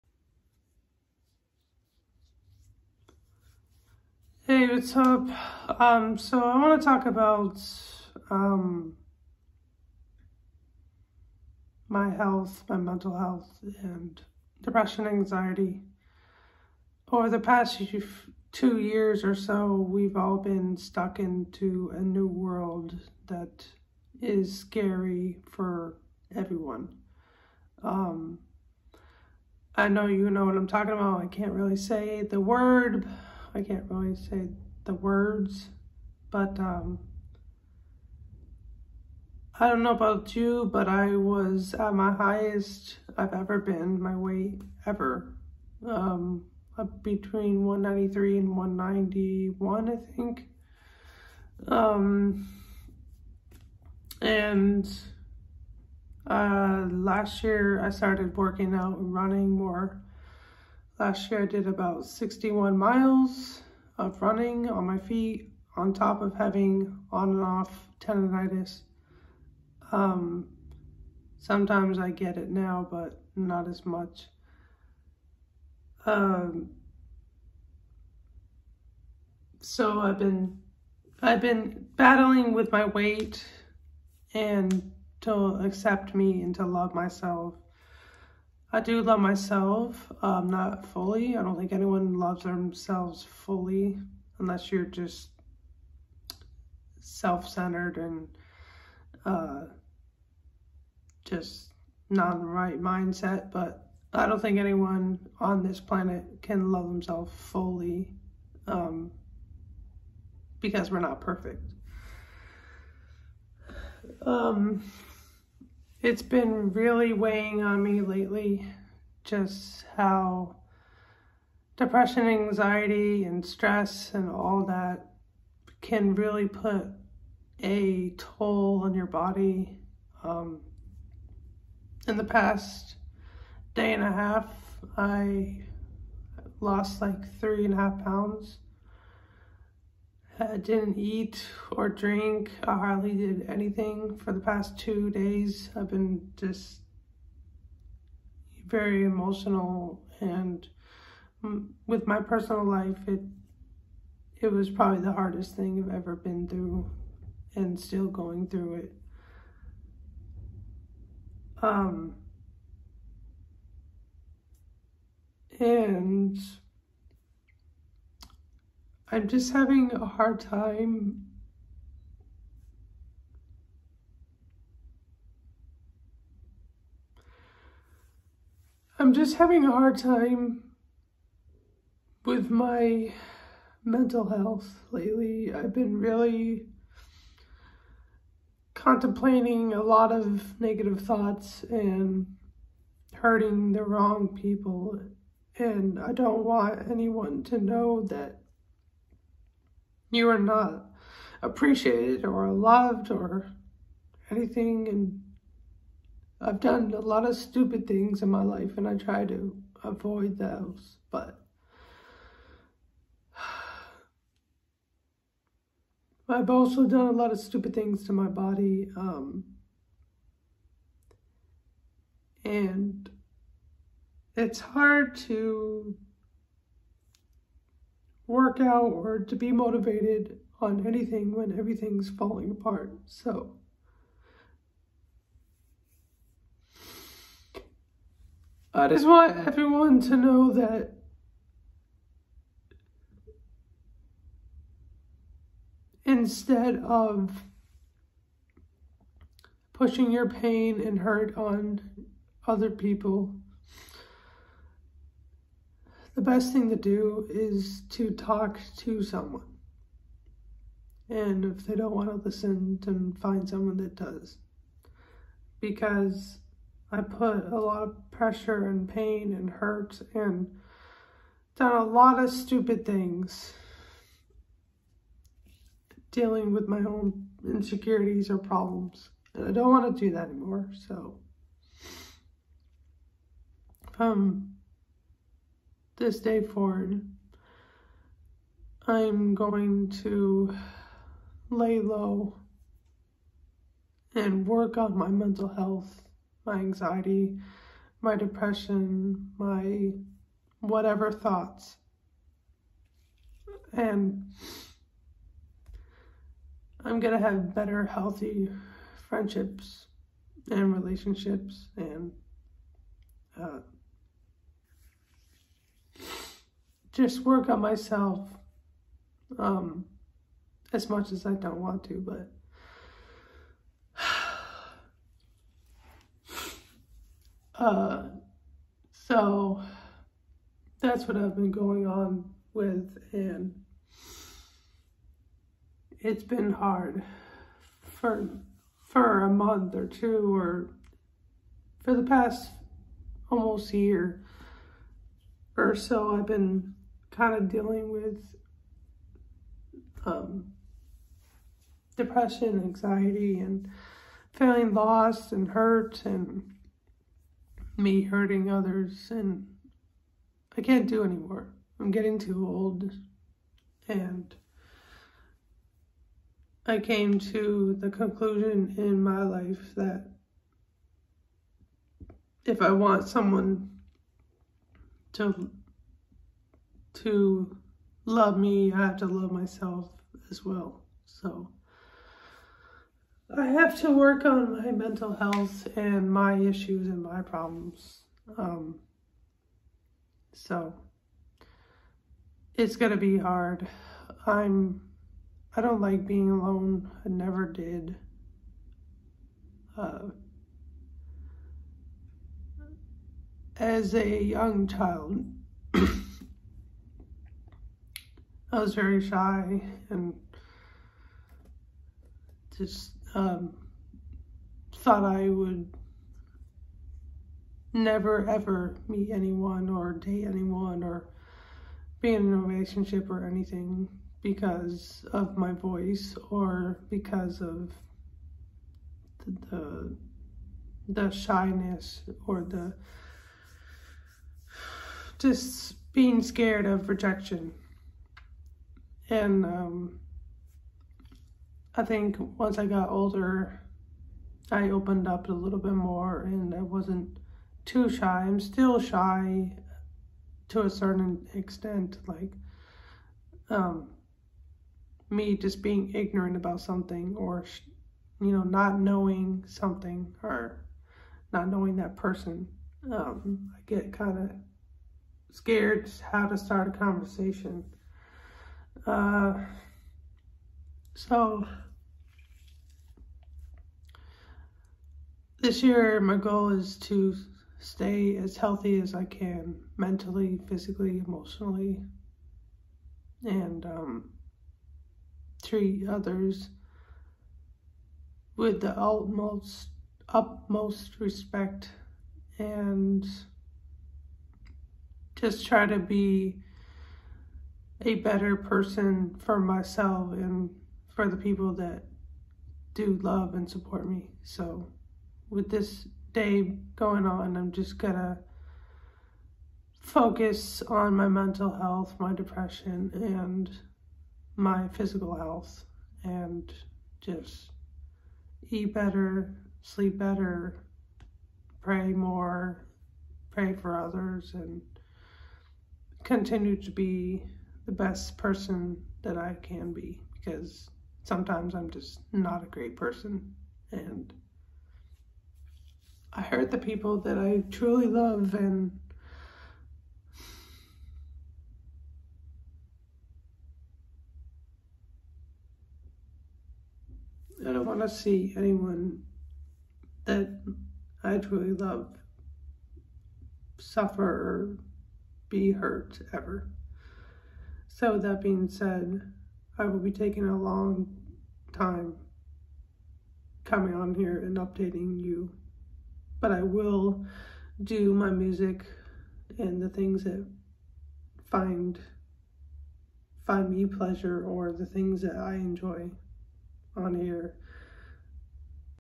My mental health, my weight ect.
A lot has happened in two years and I wanted to share if you care at all. About my personal life a bit . And how I can work on myself and love others for the good and bad. I make stupid mistakes and this time I can’t do it anymore. I need to love others regardless of my pain or my past. Or past with them. I need to focus more on getting my life is a better healthy way.